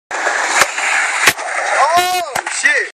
8 stair
My first 8 stair on a freezing balls day.

Dont hate